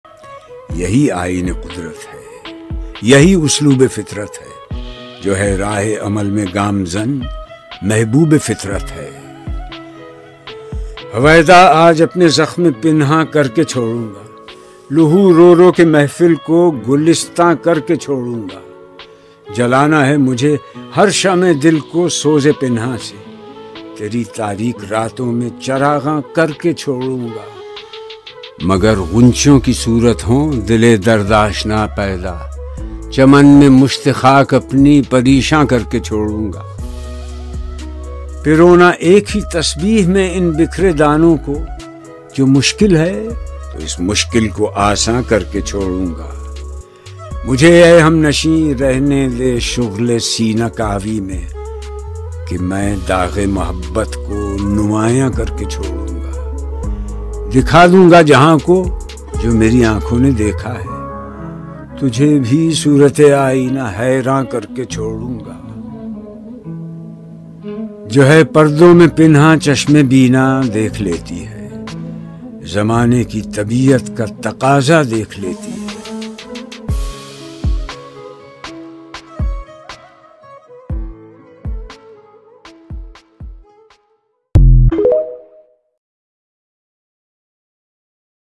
Iqbal Poetry and Imran Khan The Great in the sweat voice of late Zia Mohiuddin .یہی آئین قدرت ہے
Iqbal Poetry and Imran Khan The Great in the  sweat voice of late Zia Mohiuddin .
یہی آئین قدرت ہے یہی اسلوب فطرت ہے
اقبال کی کمال شاعری